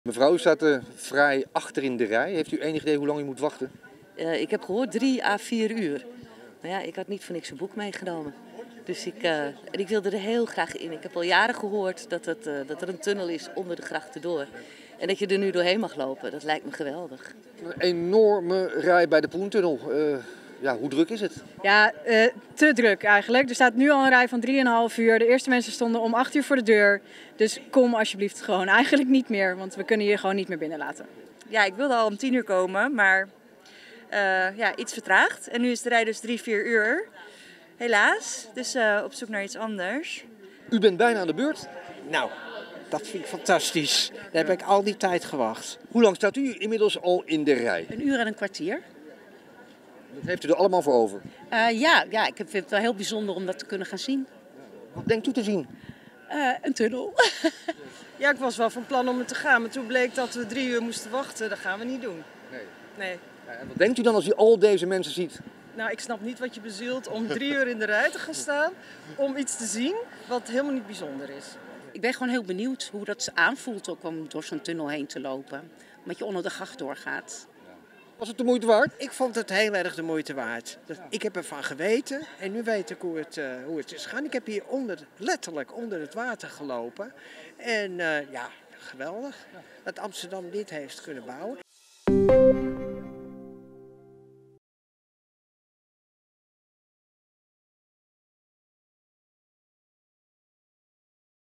Extreme belangstelling: bezoekers wachten ruim 3,5 uur voor Amsterdamse Poentunnel
Lange rijen wachtende bezoekers zondagochtend voor de Poentunnel in Amsterdam. Het is de tweede dag dat het publiek zich in groten getale verzamelt voor het monument. Gisteren trok de tunnel zo'n 1500 bezoekers. Waar de rij gisteren al zo'n twee uur duurde, staat er nu al een twee keer zo'n lange rij tot aan de Spiegelstraat met een wachttijd van meer dan 3,5 uur. De organisatie adviseert dan ook niet meer te komen.